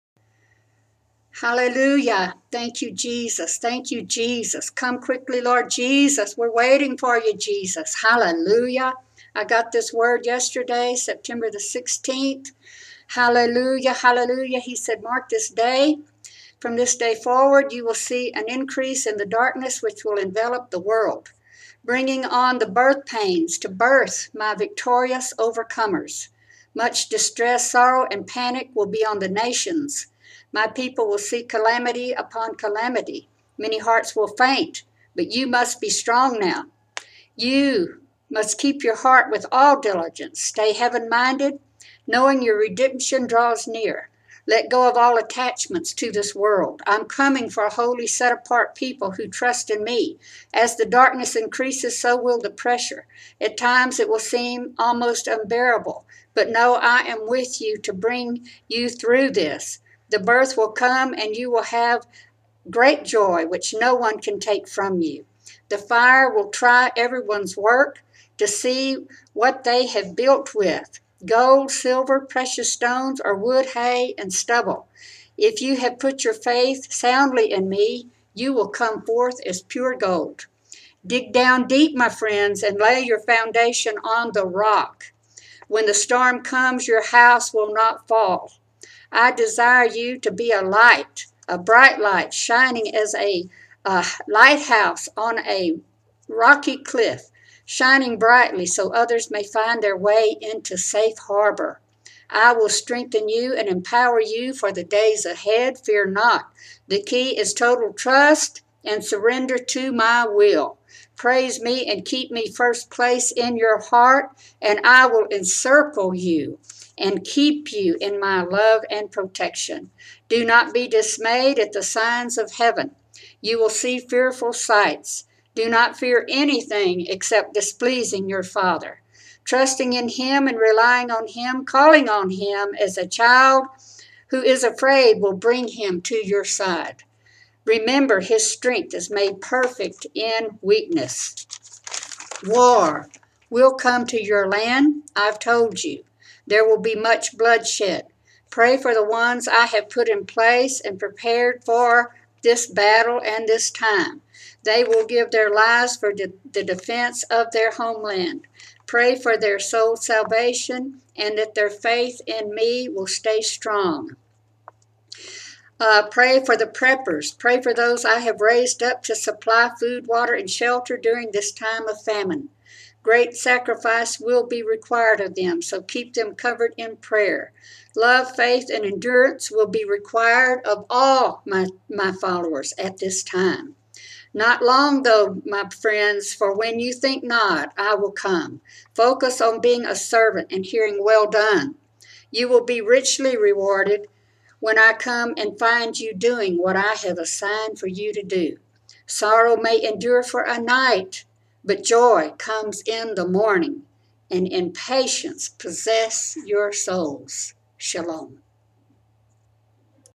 Birthing the Overcomers!
Transcript: Hallelujah! Thank You, Jesus. Thank You, Jesus. Come quickly, Lord Jesus. We’re waiting for You, Jesus. Hallelujah! I got this Word yesterday, September 16, 2014. He said…

“Mark this day. From this day forward you will see an increase in the darkness, which will envelop the world, bringing on the birth pains, to birth my victorious overcomers.
Much distress, sorrow and panic will be on the nations. My people will see calamity upon calamity. Many hearts will faint but you must be strong now. You must keep your heart with all diligence. Stay heaven-minded, knowing your redemption draws near. Let go of all attachments to this world. I’m coming for a holy, set-apart people who trust in Me.
As the darkness increases, so will the pressure. At times, it will seem almost unbearable. But know I am with you to bring you through this. The birth will come and you will have great joy which no one can take from you. The fire will try everyone’s work to see what they have built with: gold, silver, precious stones or wood, hay and stubble. If you have put your faith soundly in Me, you will come forth as pure gold. Dig down deep, my friends, and lay your foundation on the Rock. When the storm comes, your house will not fall.

I desire you to be a light, a bright light, shining as a lighthouse on a rocky cliff -- shining brightly so others may find their way into safe harbor. I will strengthen you and empower you for the days ahead. Fear not! The key is total trust and surrender to My will. Praise Me and keep Me first place in your heart and I will encircle you and keep you in My love and protection. Do not be dismayed at the signs of heaven. You will see fearful sights. Do not fear anything except displeasing your Father. Trusting in Him, and relying on Him, calling on Him (as a child who is afraid) will bring Him to your side. Remember, His strength is made perfect in weakness.

War will come to your land, I’ve told you. There will be much bloodshed. Pray for the ones I have put in place and prepared for this battle and this time. They will give their lives for the defense of their homeland. Pray for their soul’s salvation and that their faith in Me will stay strong. Pray for the preppers. Pray for those I have raised up to supply food, water and shelter during this time of famine. Great sacrifice will be required of them, so keep them covered in prayer. Love, faith, and endurance will be required of all my followers at this time. Not long, though my friends, for when you think not, I will come. Focus on being a servant and hearing, “Well done!” You will be richly rewarded when I come and find you doing what I have assigned for you to do. Sorrow may endure for a night but joy comes in the morning; and in patience, possess your souls. Shalom.”